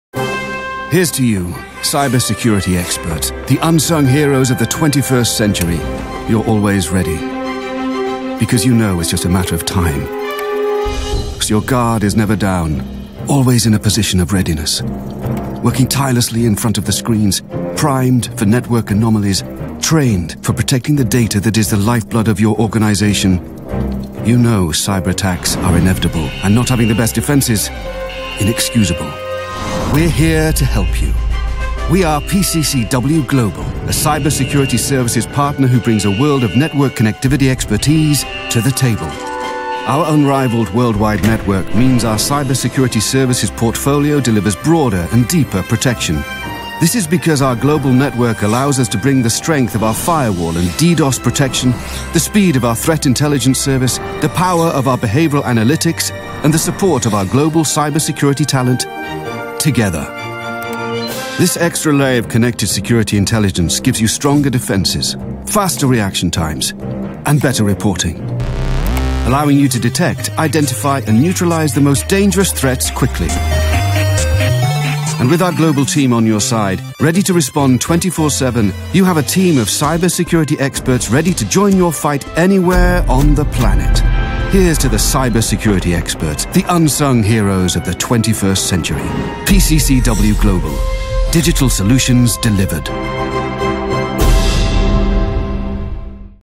PCCW Global Cyber Security: The unsung heroes of the 21st century
At PCCW Global we believe that the right talent with the right tools is vital battling cyber criminals.

For more information about how you can partner with us and make your Cyber Security offering better, faster, stronger